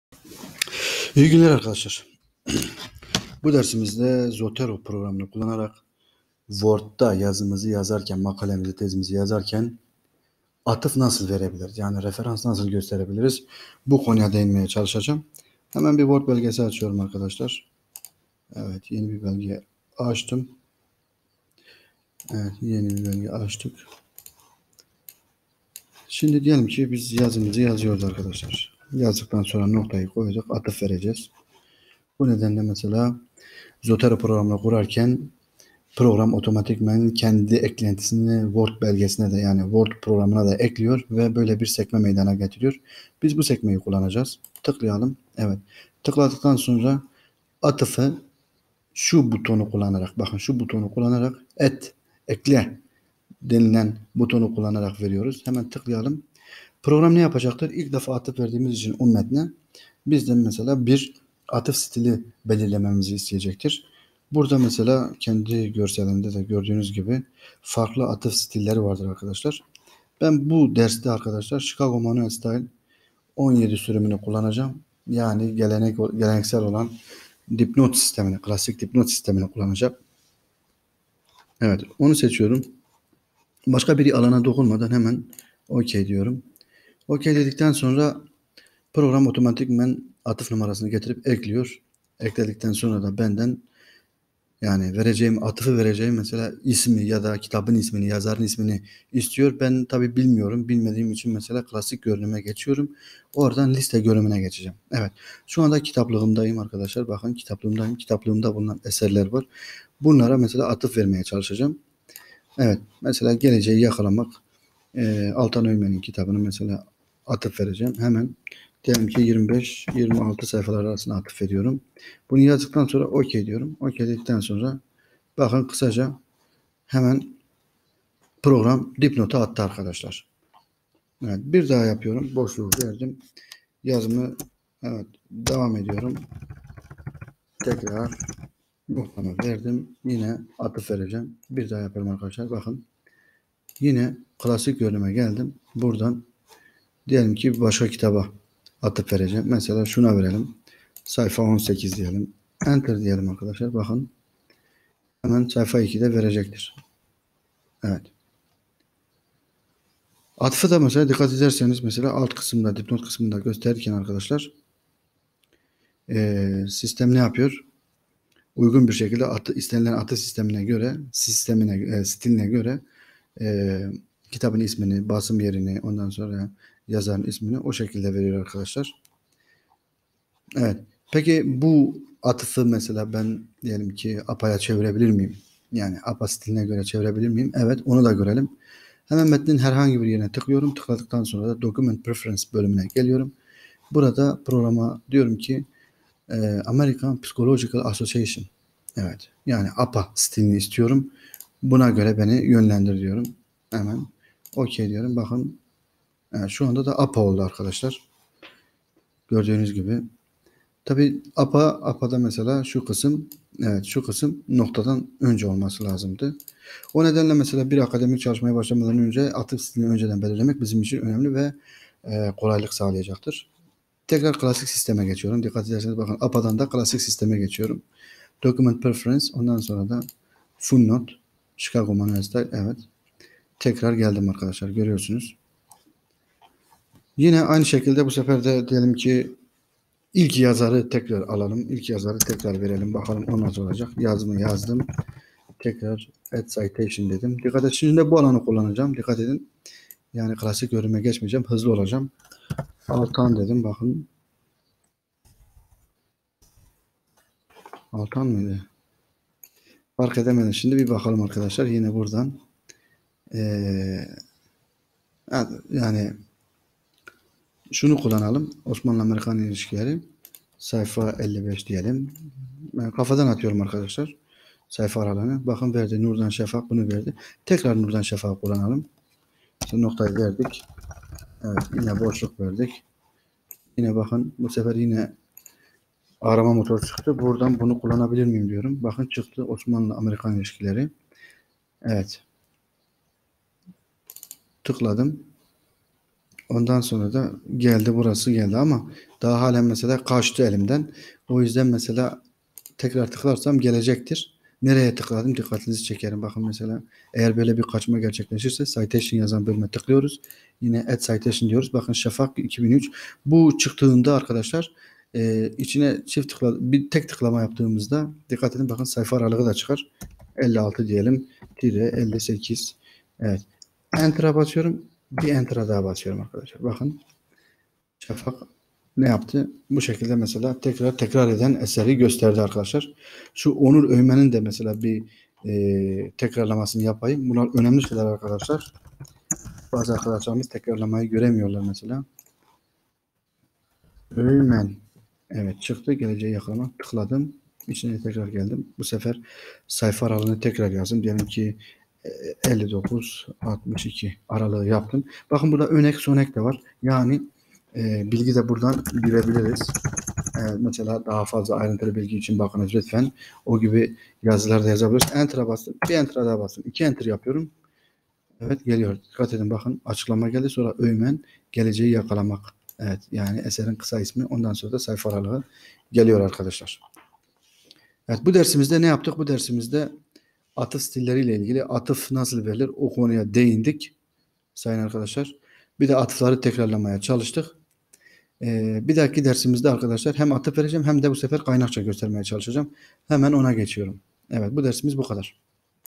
Ders 24 Zoteroda Atıf stili belirleme
diğer dersler bu video serisinde akademik yazımın bir diğer önemli programı olan zotero hakkında ders videoları hazırlayarak programın öğrenilmesini amaçladım.